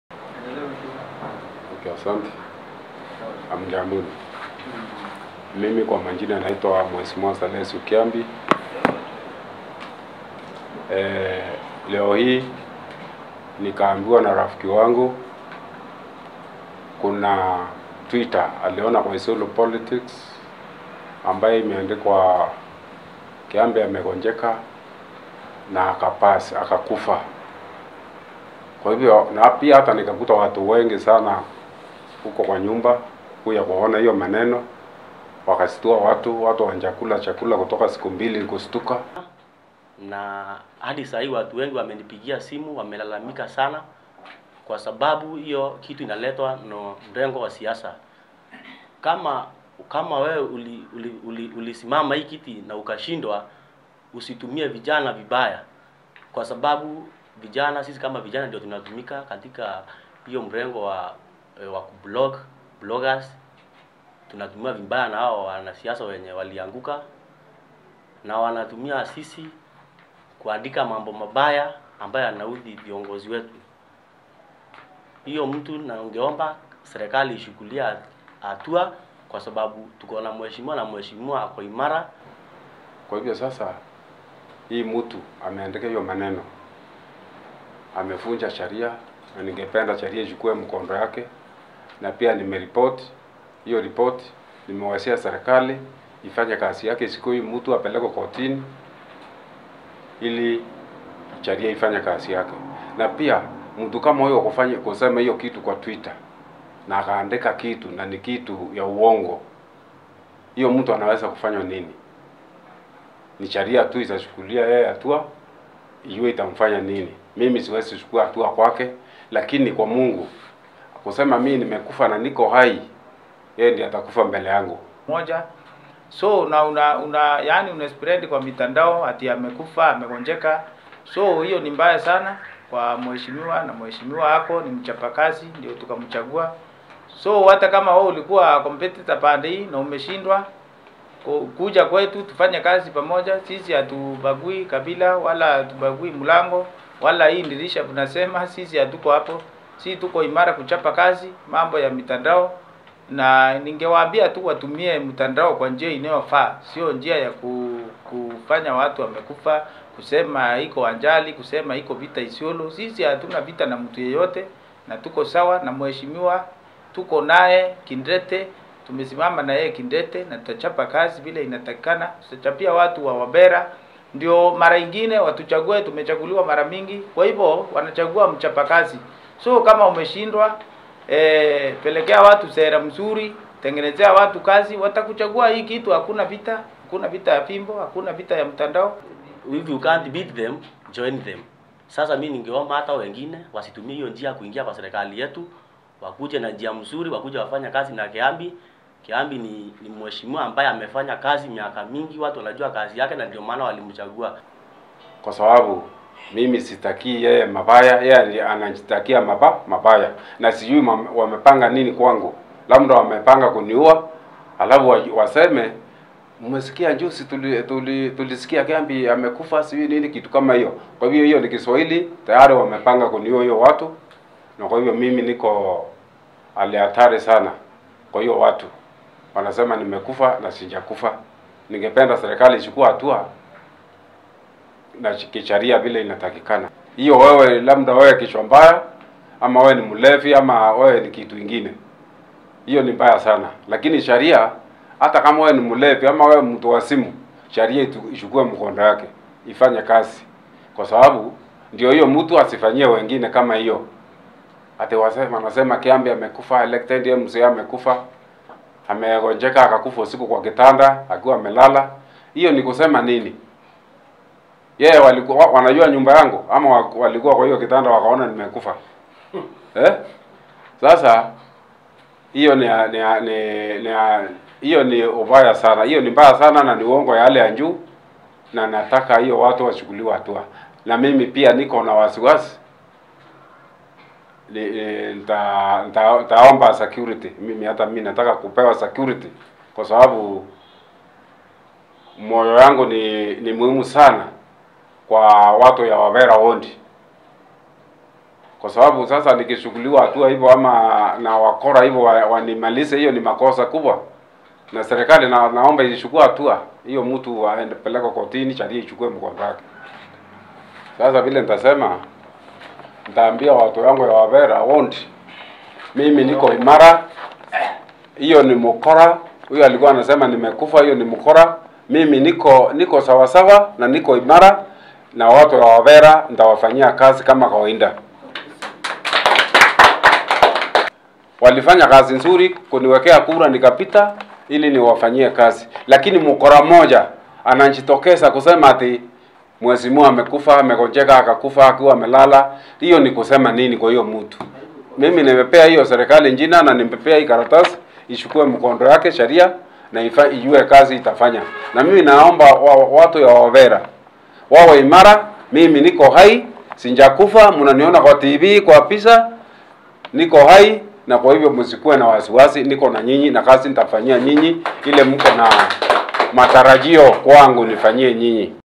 "I am not Dead," Salesio Kiambi MCA Wabera now says after viral tweet
Salesio Kiambi MCA elect Wabera ward has opened charges against a blogger who claimed he was dead on Twitter